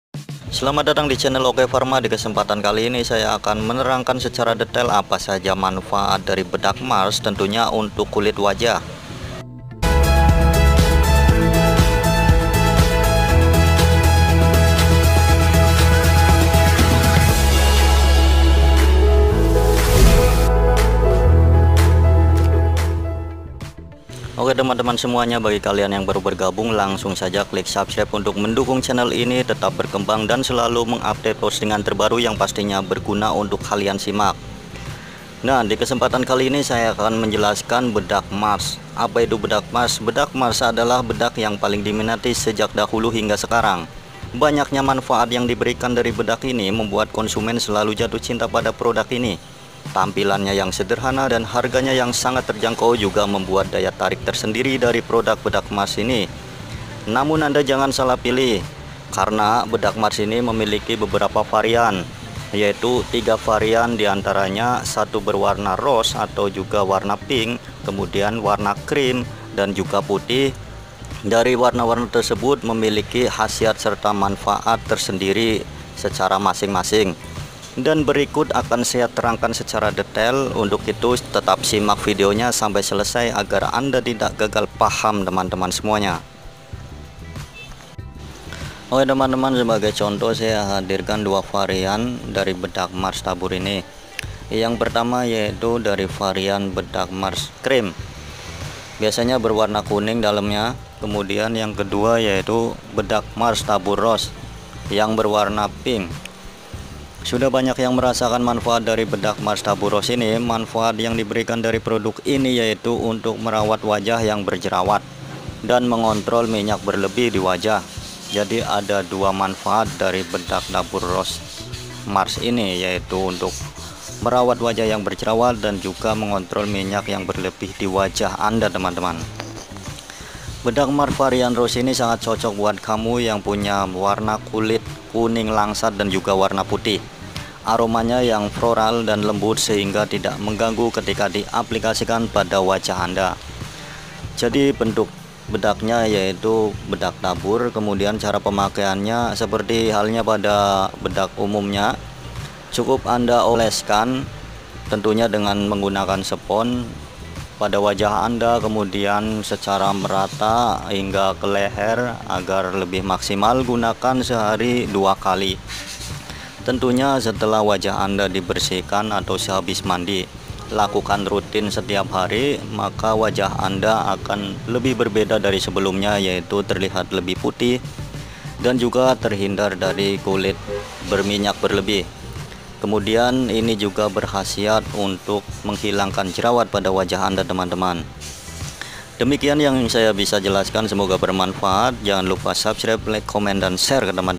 Manfaat bedak marcks rose benarkah bisa atasi jerawat
Bedak marcks memiliki berbagai varian
Kali ini saya review bedak marcks rose
Bedak tabur marcks dipercaya memiliki
Banyak manfaat untuk kulit wajah
Diantaranya mengatasi kulit wajah
Berjerawat dan mengontrol kulit wajah berminyak.
Salah satu varian bedak marcks rose
Ini bermanfaat untuk wajah berjerawat
Dan kulit wajah berminyak